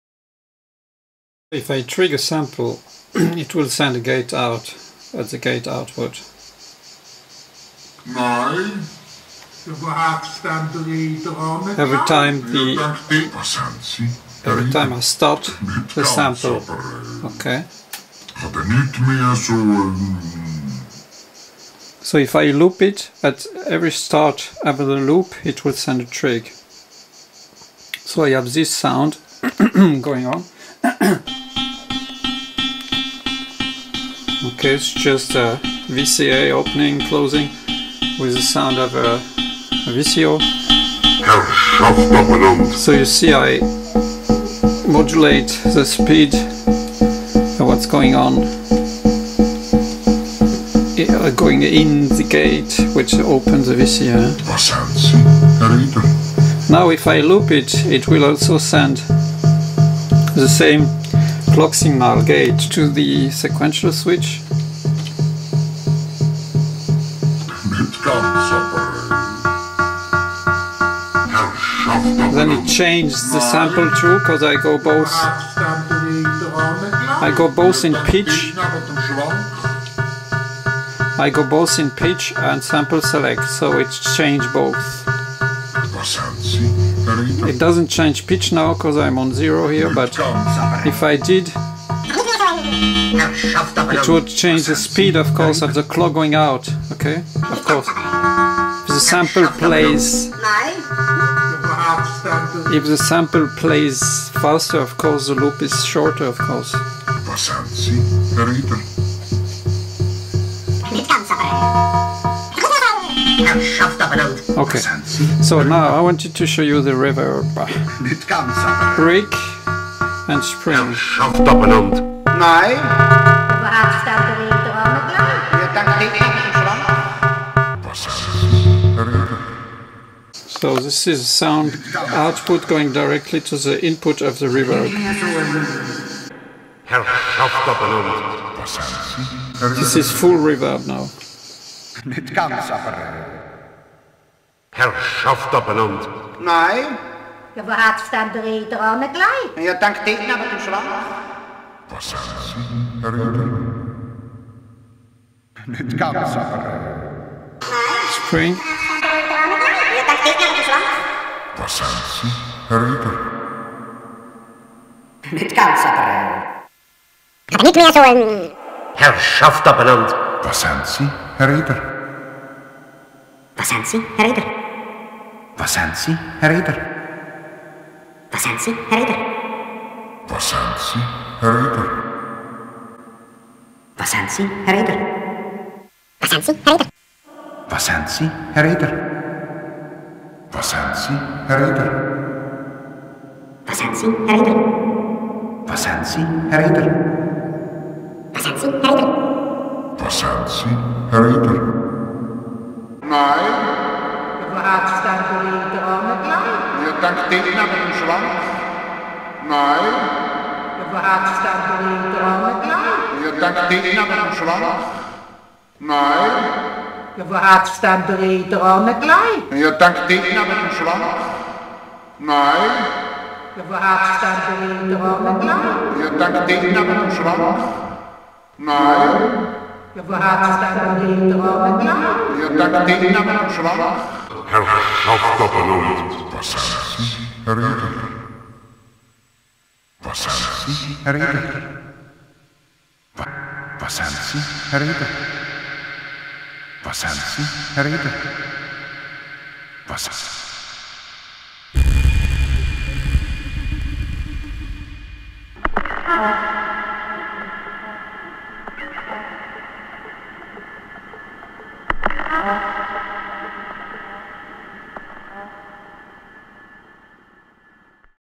10 08 2020 Késako Player & Reverb [03]
Playing with REVERB.

Sorry for the quality of the very few videos I’m posting since a couple of months.
The internet connection is miserable here in despite of paying a high price.
Upload speed: 20kbps (!).
Orange France has the monopole here and they do what they want.

I just ordered new PCBs and panel for the new proto.
I will of course make proper demos when the module is ready and upload  in HD from a different place.

The pots mounted on this proto are not the final ones, that’s why turning the knobs looks lousy.